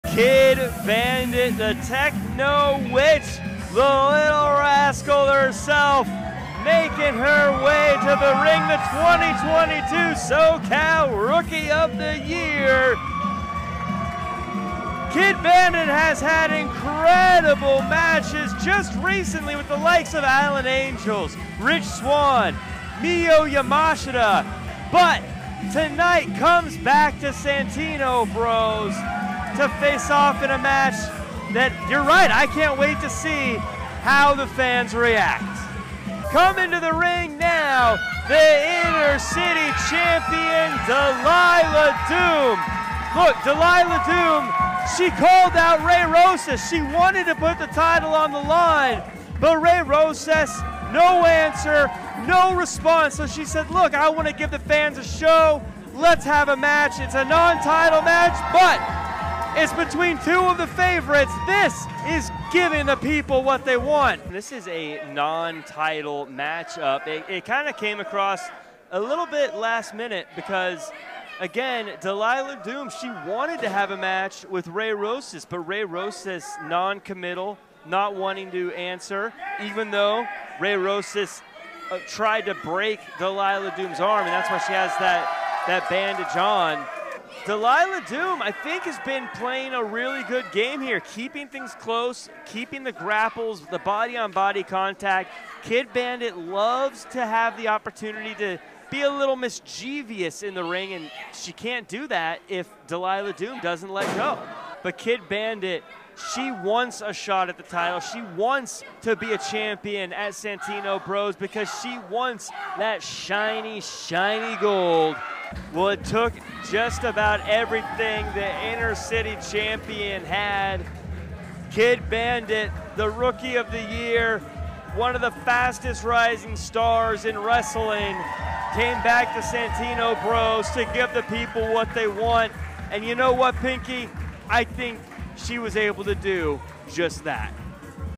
SANTINO BROS | FIGHT NIGHT 9 | Kidd Bandit vs Delilah Doom Highlights
Follow Santino Bros for more!